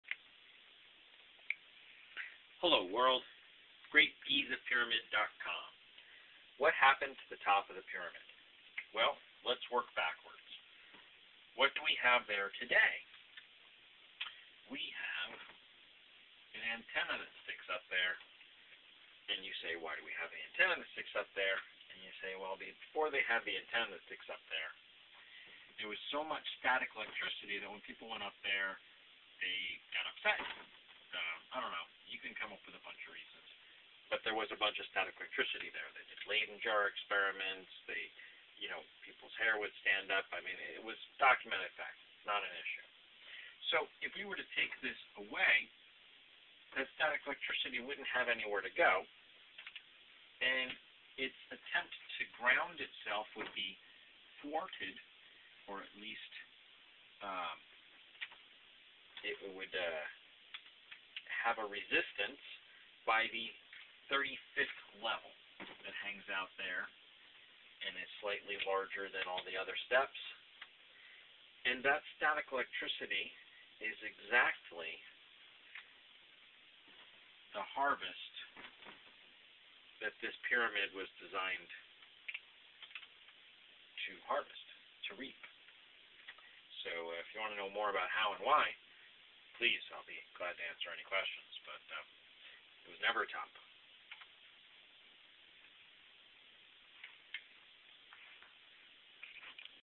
There was never a capstone on top - Great Giza Pyramid (com)
The designers of the pyramid used many natural features of the elements to make the pyramid run. I hope you can follow along, I will be glad to answer any questions.

contact at Great Giza Pyramid dot com

657.229.GIZA(4492) voice/sms/text (NO MMS/photos at this time)

Hyperlinks to the images used can be found on the website, or if this is an old video by request.